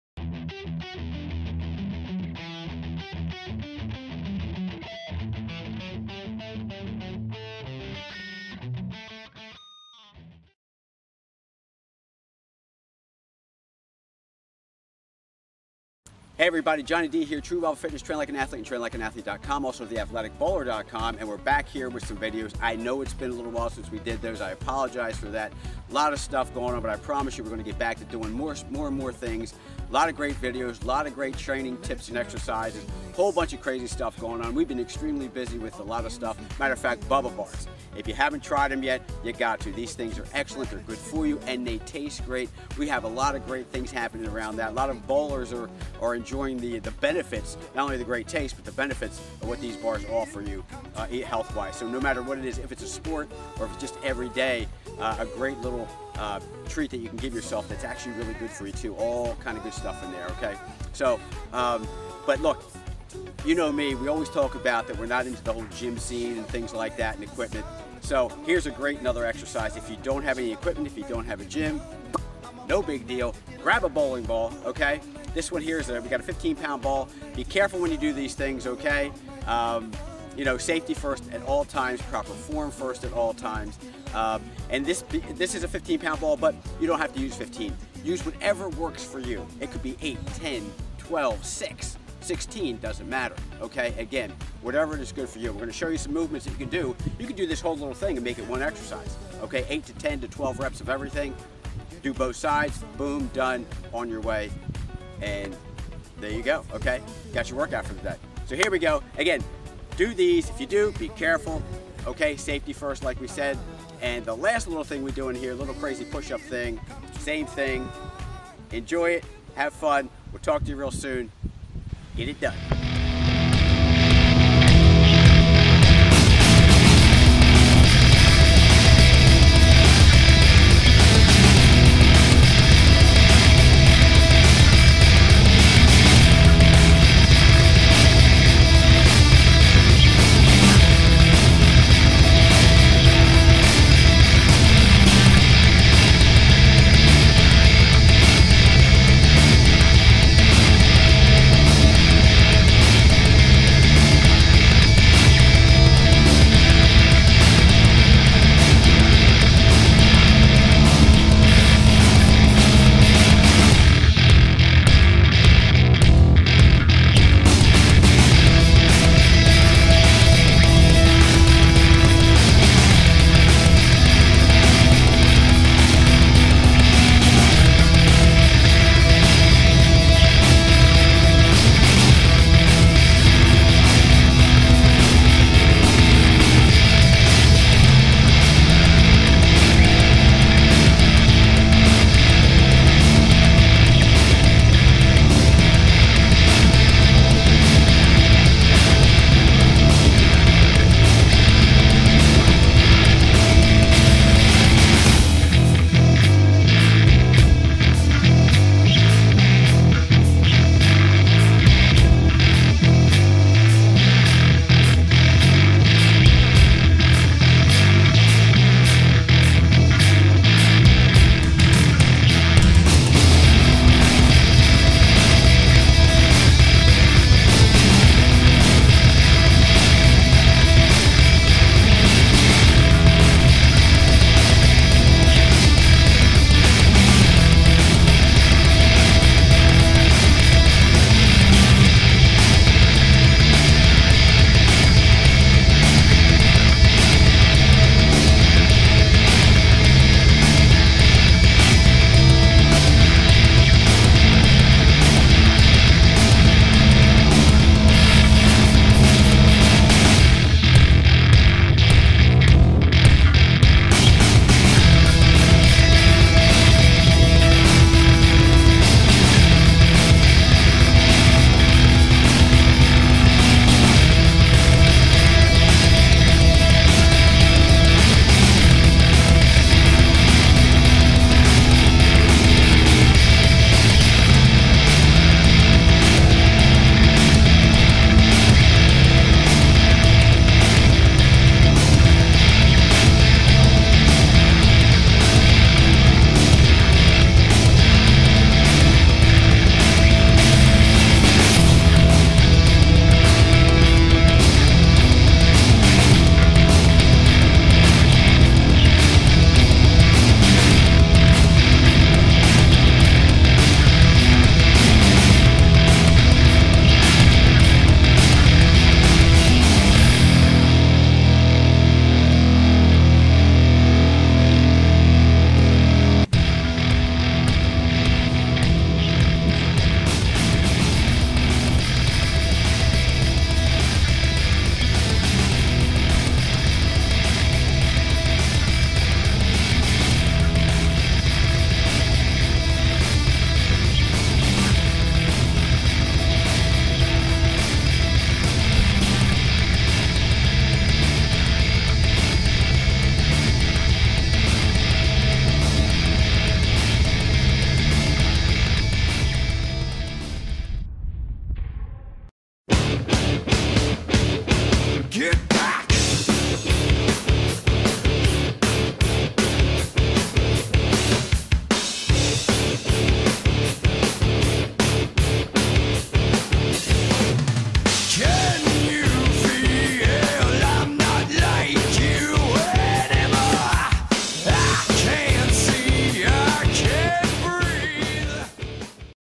Crazy sh*t to do with a bowling ball (Part 1)
Crazy Stuff To Do With A Bowling Ball (Part 1)

Here is part 1 of a series of movements that if put together will make one great workout. No gym or equipment needed. The only thing you’ll need is a bowling ball. (Can also be done with a Med ball). Just do 8-12 reps of each move. Do both sides if it calls for it. Repeat 2-3 times or, just one set if that’s all the time you have. Work hard, work smart, and have fun.